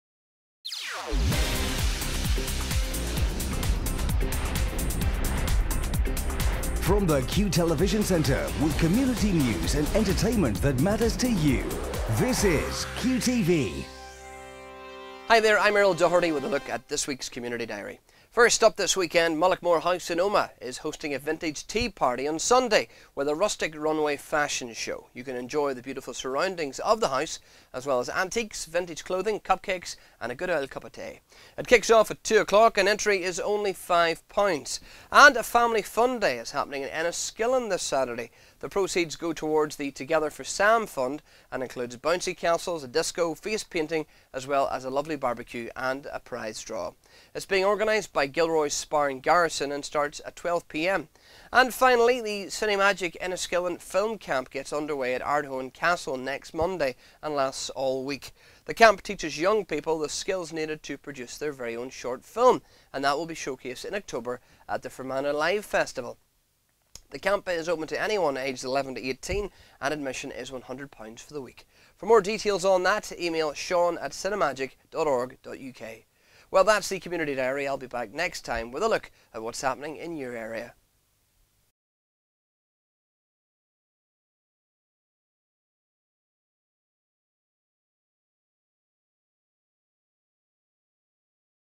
Community Diary for the Week 8 August
Errol from Q Breakfast takes a look at the week's Community Diary.